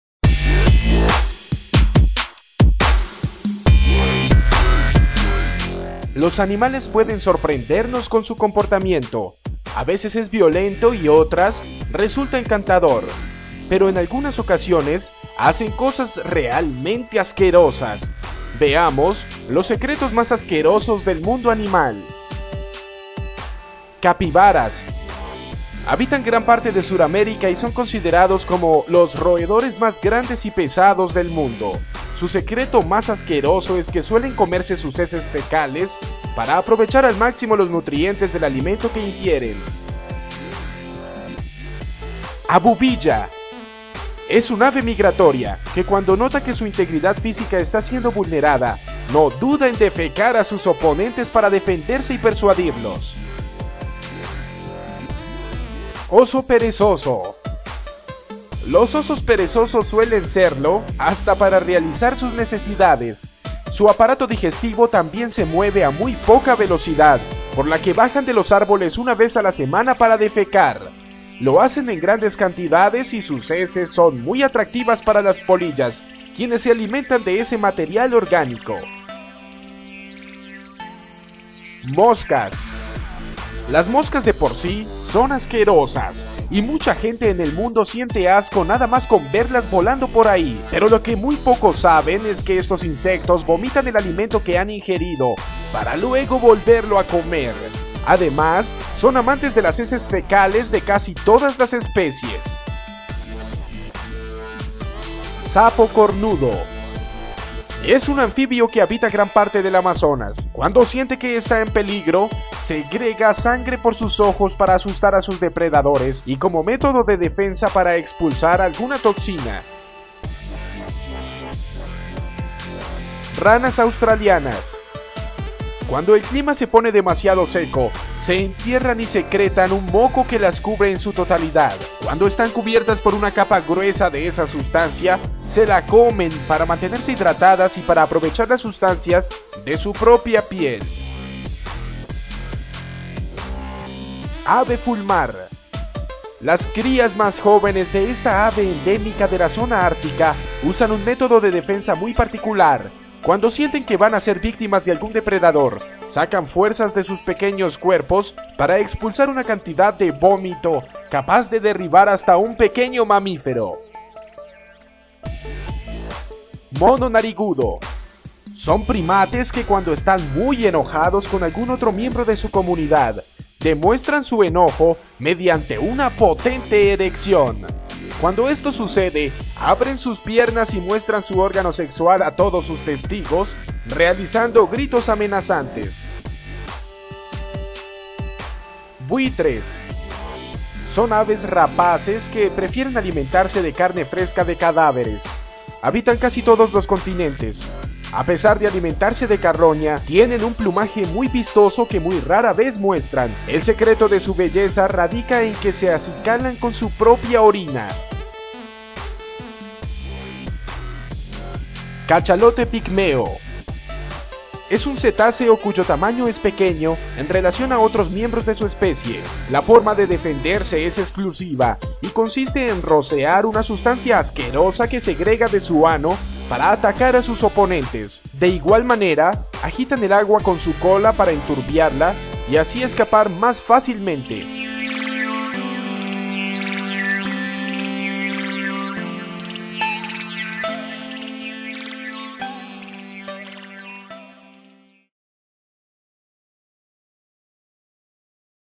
Las cosas más desagradables que hacen los animales
Los animales hacen algunas cosas que no son nada ortodoxas y resultan bastante grotescas y asquerosas, en la mayoría de veces ante sus adversarios y como mecanismo de defensa, en otras ocasiones para demostrar su enfado, como mecanismo de alimentación y supervivencia, etc.

El contenido de este video es de carácter educativo | Las imágenes podrían ser solamente ilustrativas y no corresponderse con la realidad | Producido por tutovariedades.